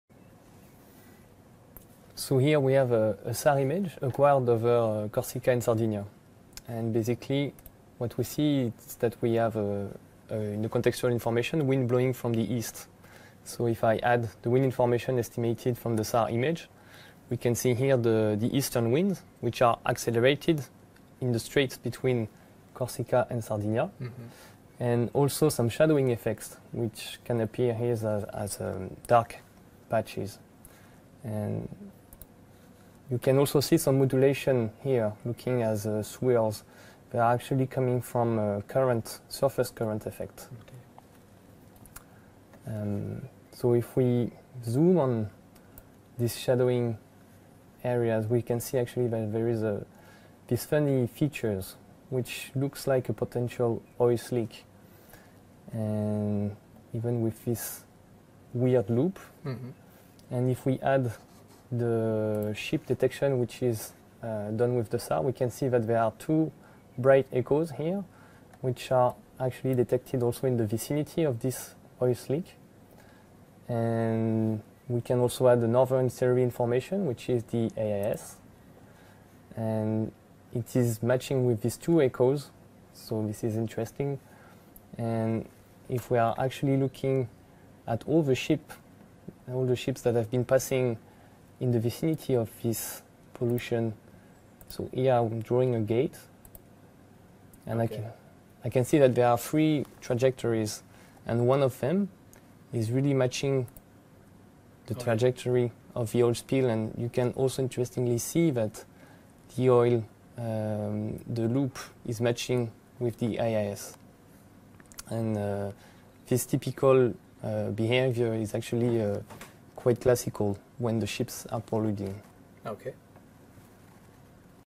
ESA Echoes in Space - Water: NRT Oil Spill mapping Example I
Romain Husson gives an example of the Oil Spill Mapping service provided by CLS